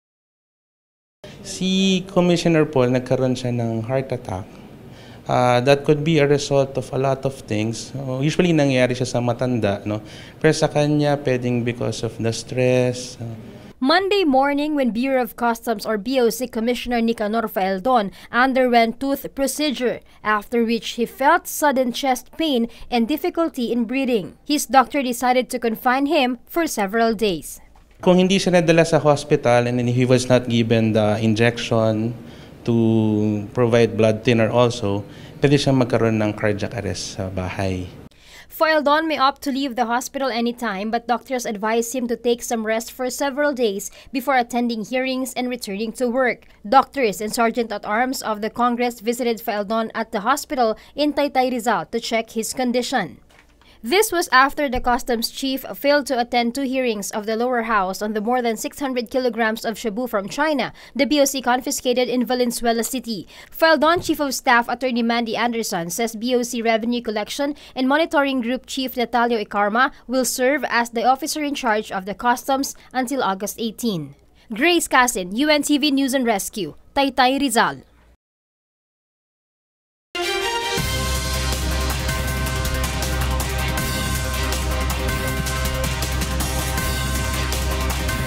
BOC Commissioner Nicanor Faeldon, now in stable condition after severe chest pain due to stress
Bureau of Customs Commissioner Nicanor Faeldon is now in good condition following hospital confinement